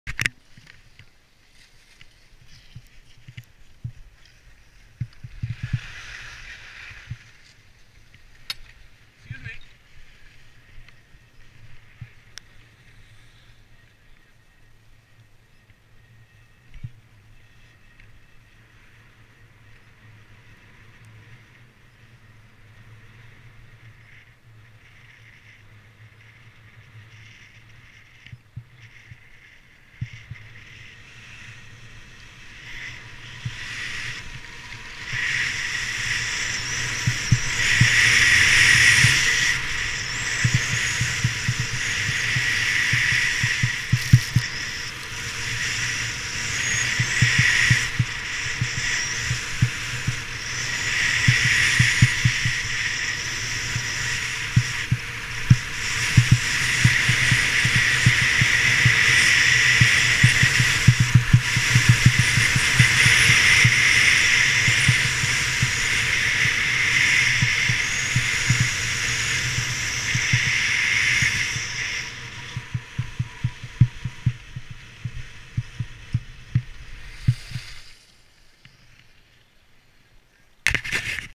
Big Powderhorn Mountain Resort - December 27, 2015
For those who couldn't be here today, this is what you missed.  For everyone else, hope you enjoyed your day!